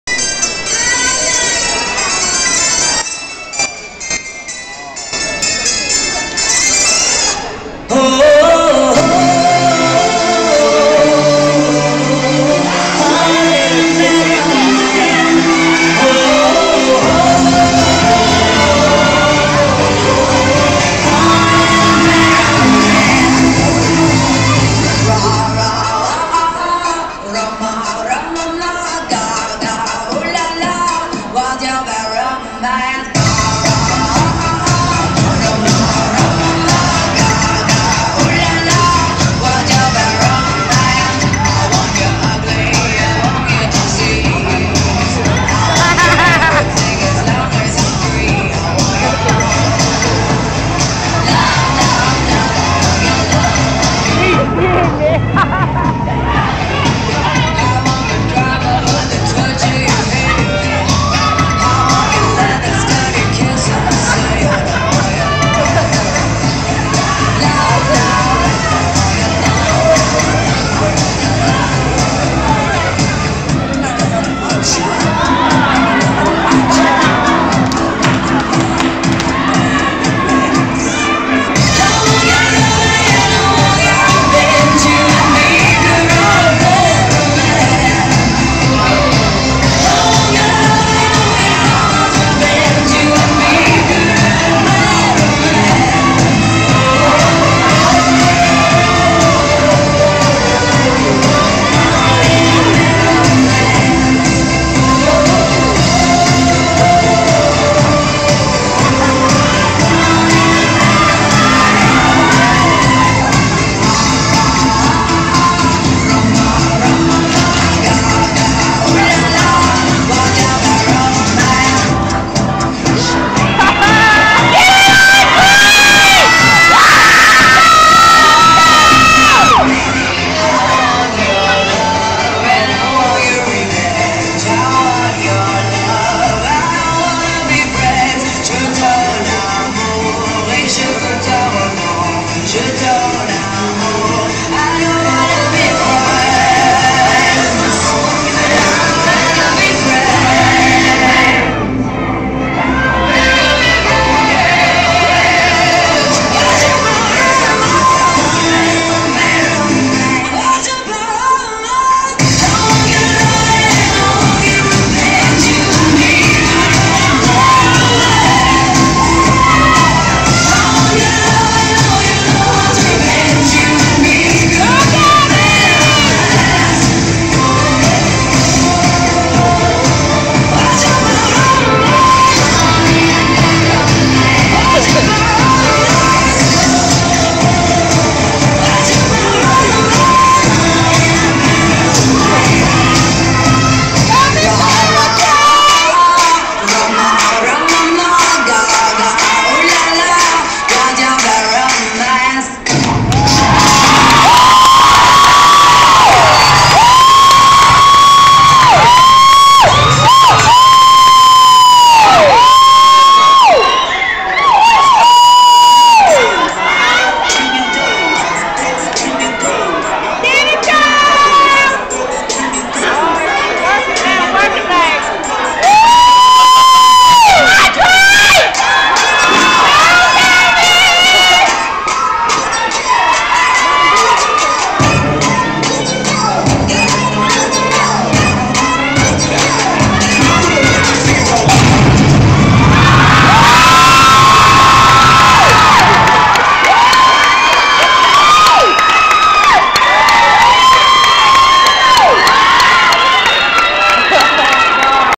"Ain't No Half Steppin'" Step Compeitition (2/27/10)--HighStepper Performance of Lady GaGa
HighStepper performance of Lady GaGa at the 1st Annual Westview Middle School step compeitition.